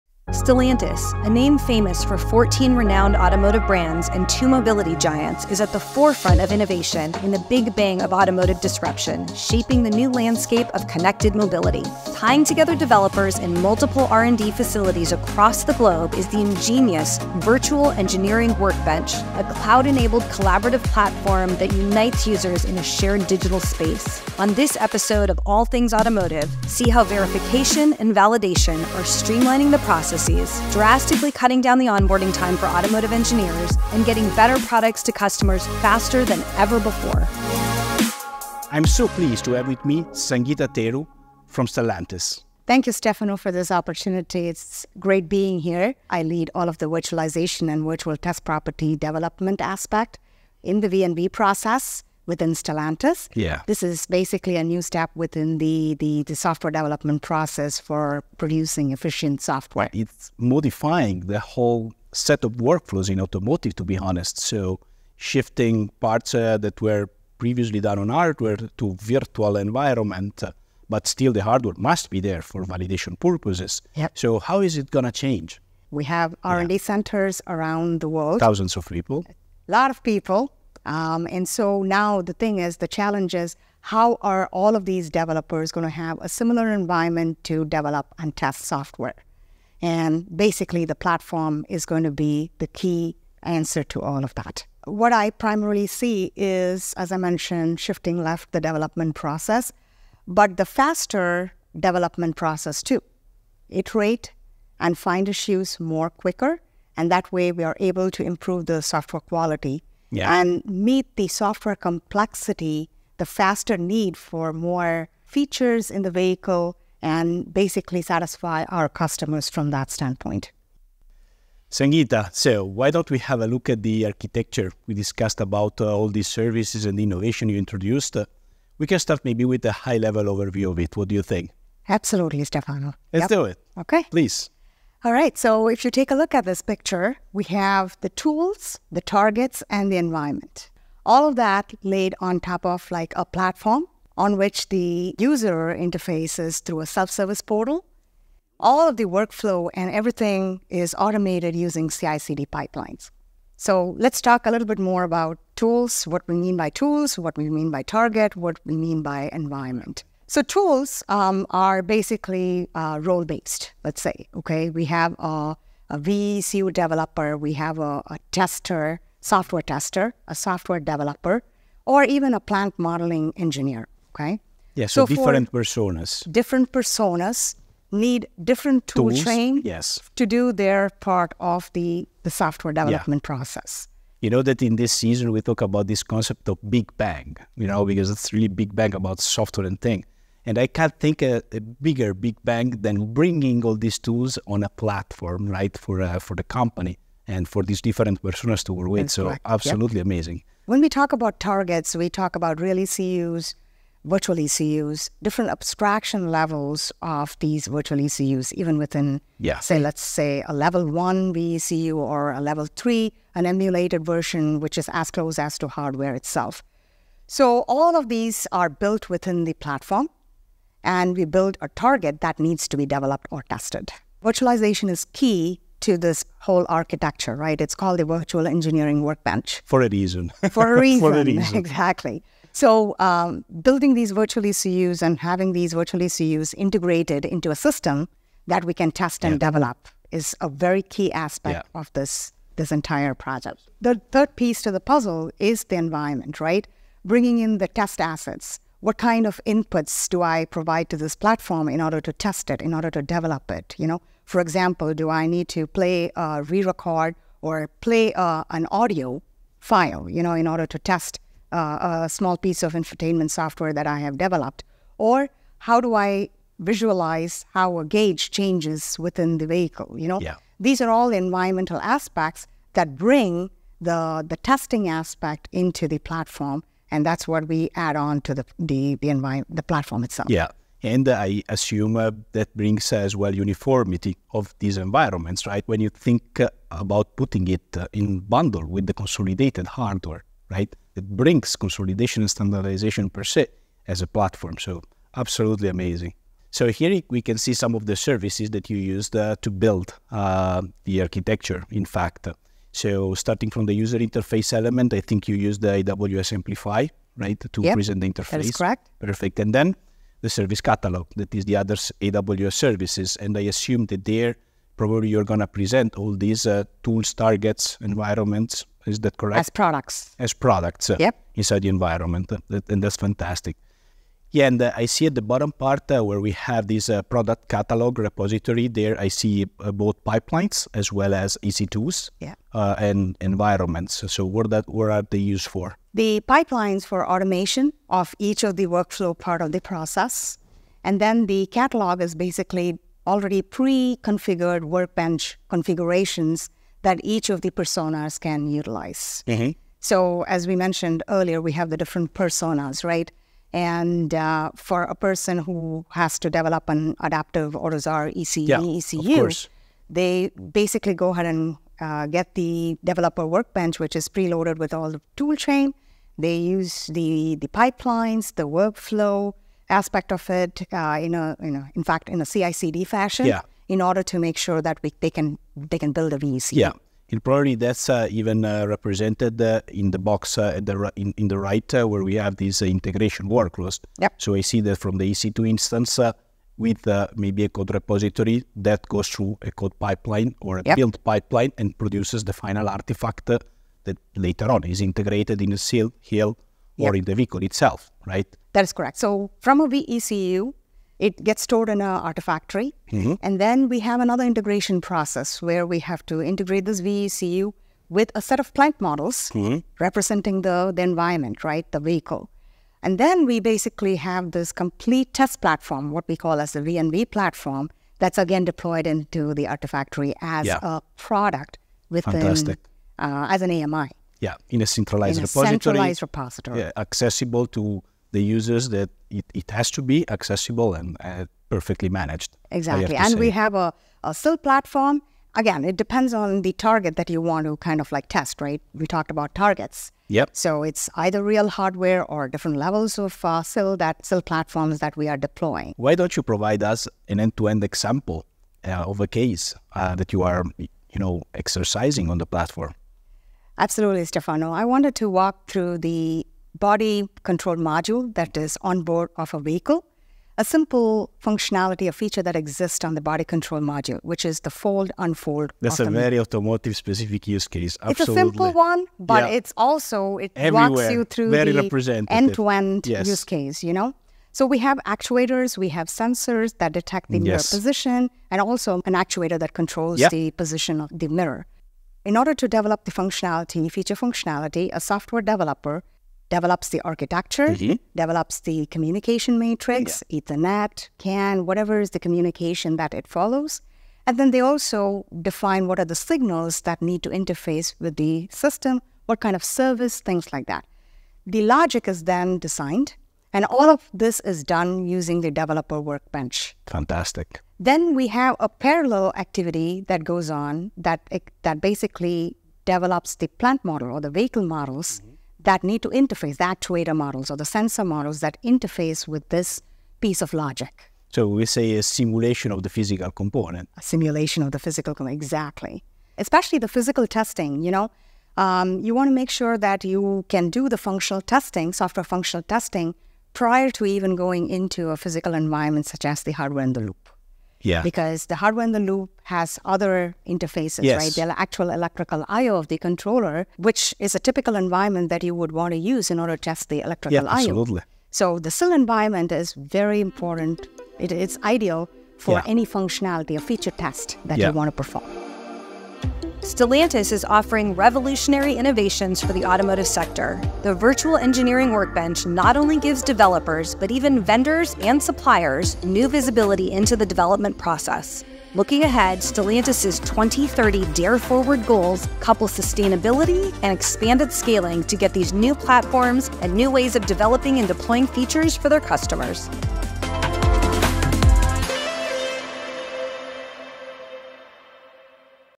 Stellantis’ Virtual Workbench for Automotive Software Evolution and Connected Mobility
Experience the evolution of software development practices in automotive on this episode of All Things Automotive, with special guest Sangeeta Theru, Sr. Manager for Stellantis’ Verification & Validation Platform.

Together with host Stefano Marzani, Worldwide Technical Lead for AWS, you'll get a peek at Stellantis' pioneering innovation, the Virtual Engineering Workbench. This self-service platform for thousands of engineers brings together automotive tools, targets, and environments on a cloud-enabled platform. Explore how it's optimizing the verification, validation, and product delivery processes, enabling quicker launches while maintaining the highest quality standards.

Watch now and experience the big bang of automotive disruption on this special episode featuring AWS partner Stellantis.

0:00 - Introduction to Stellantis
0:38 -  Virtual Engineering Workbench Overview
1:03 -  Streamlining Verification and Validation
1:37 -  Challenges in Development Process
2:01 -  Architectural Overview of the Virtual Engineering Workbench
4:59 -  Services Used in the Architecture
7:26 -  End-to-End Example: Body Control Module
9:17 -  Stellantis' Future Goals